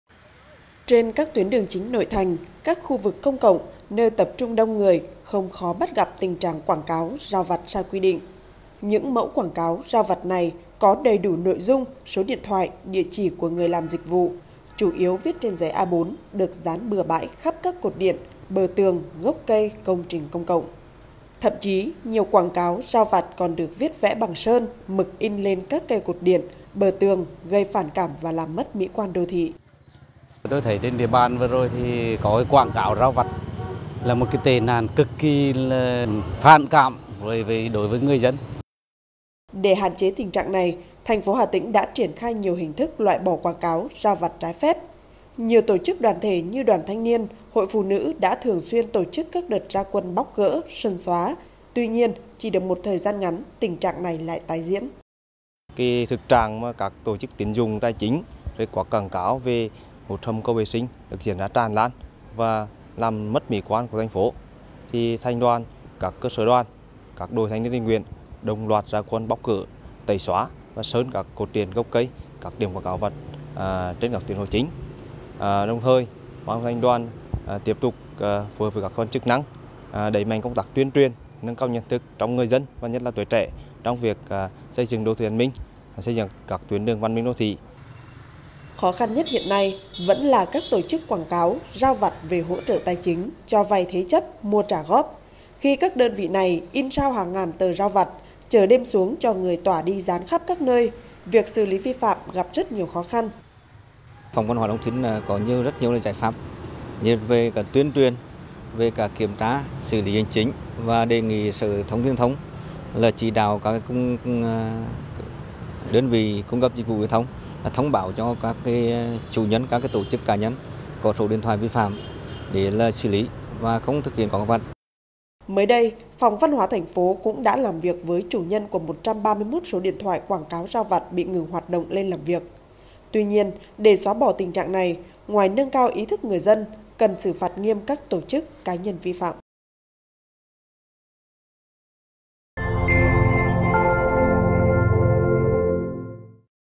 Mạnh tay "dọn rác" quảng cáo, rao vặt trái phép!
Tình trạng quảng cáo, rao vặt trái quy định trên địa bàn TP Hà Tĩnh diễn ra tràn lan, làm mất mỹ quan đô thị, gây nhiều bức xúc trong quần chúng nhân dân. Bằng nhiều biện pháp quyết liệt, thành phố đã và đang kiên quyết xử lý các trường hợp vi phạm.